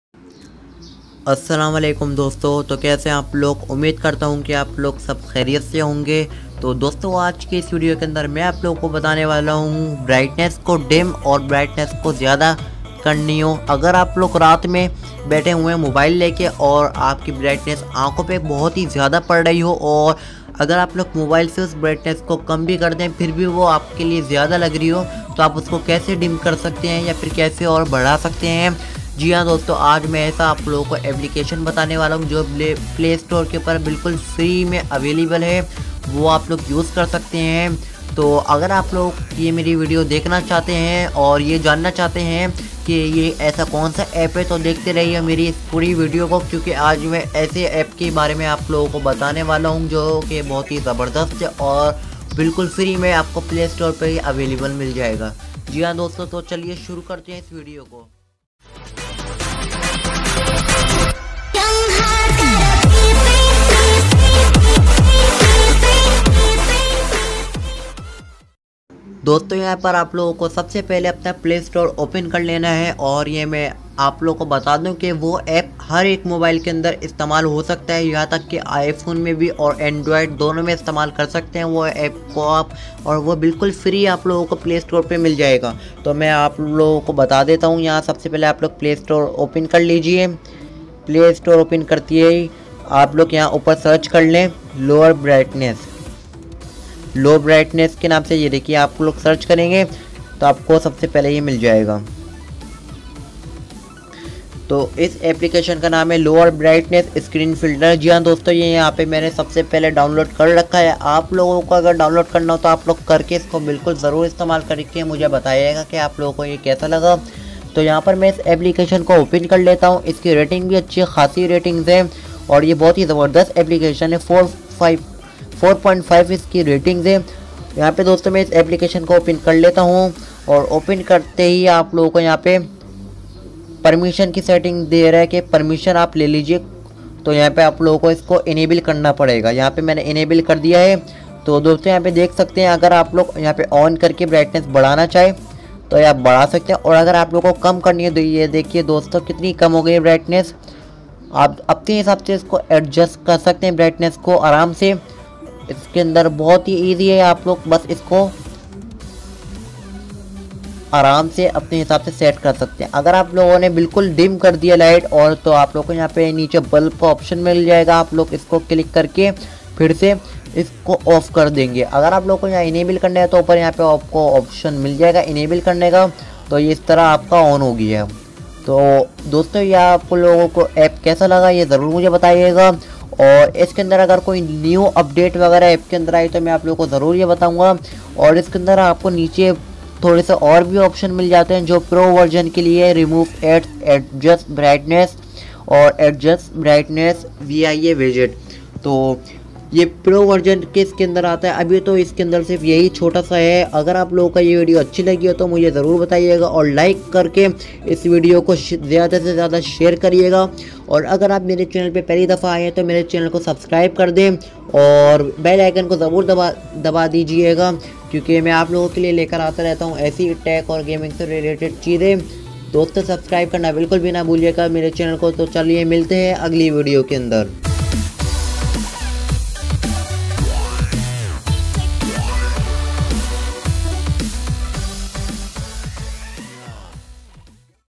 Increase or decrease your mobile brightness 2019 free
Increase or decrease your brightness
Features.
- Reduce screen brightness below the system minimum brightness
- Easy to use. Just enable and set the percent of brightness level (0-100%)
- Auto start after reboot
- Brightness icon on top notification bar (select notification bar to open brightness app setting)
- Easy to use. Just choose percent of brightness you want to set.
- Small size of application.